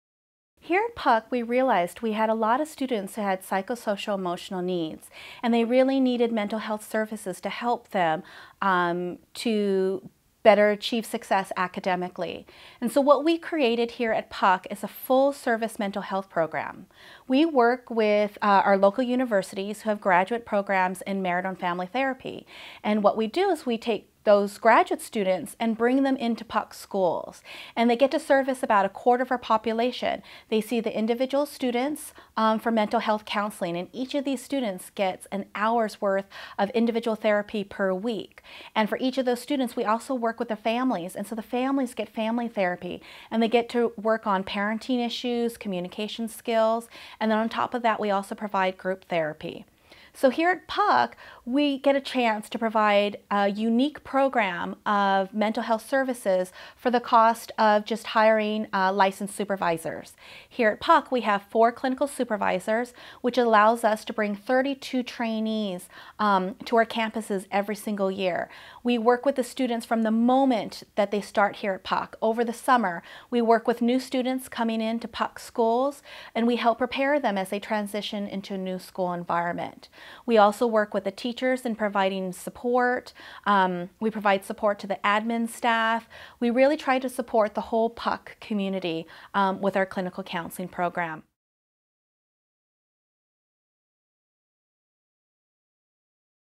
An Innovative Partnership to Provide Mental Health Services to Students
PUC Schools didn't let a tight budget stand in the way of getting students the counseling and support they needed to be successful. This network of charter schools serving low-income students in Los Angeles created a unique partnership with local universities that allows them to provide much-needed mental health services to both students and their families.